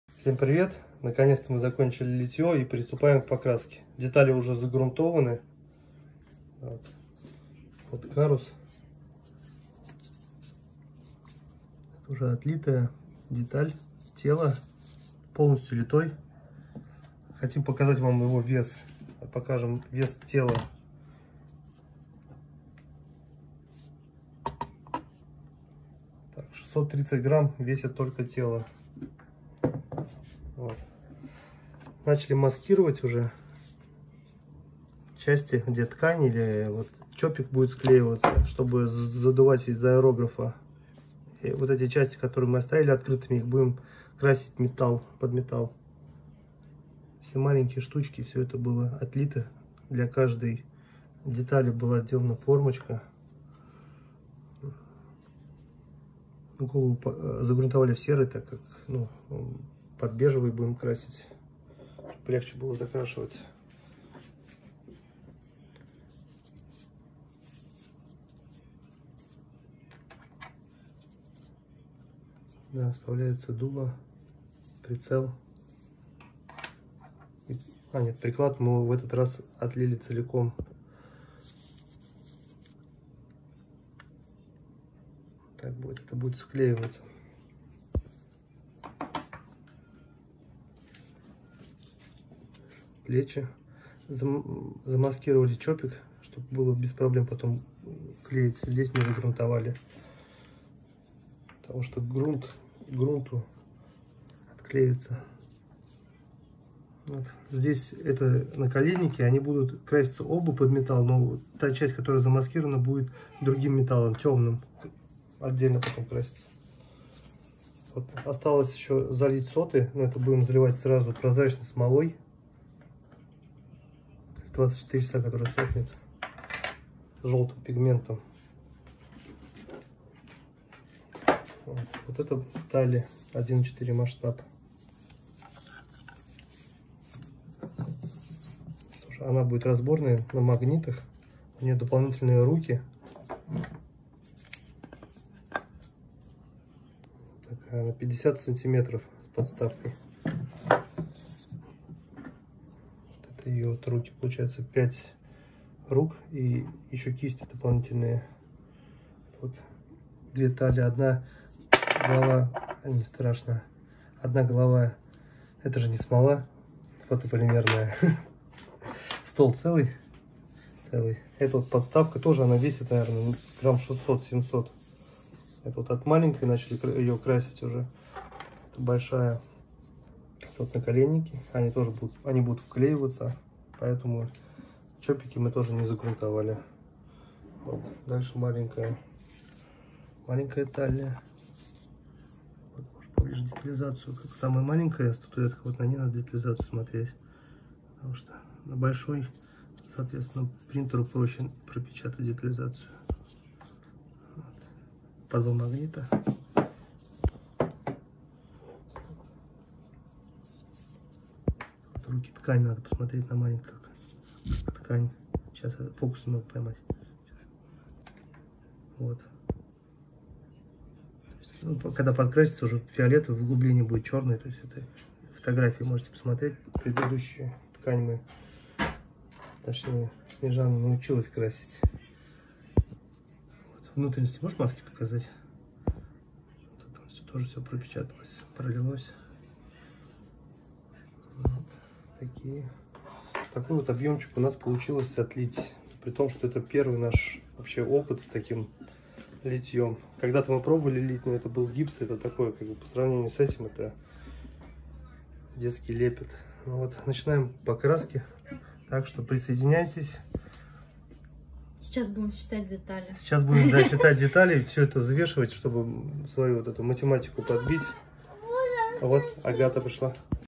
Статуэтки из полиуретана
Отлили 5 статуэток, три Гарруса в масштабе 1\6 Тали в масштабе 1\4 и 1\6. В общей сложности 123 детали, маленькая часть не попала в кадр. В течение двух месяцев изготавливали формочки. Сейчас приступаем к покраске, за которой Вы сможете понаблюдать у нас в группе ВК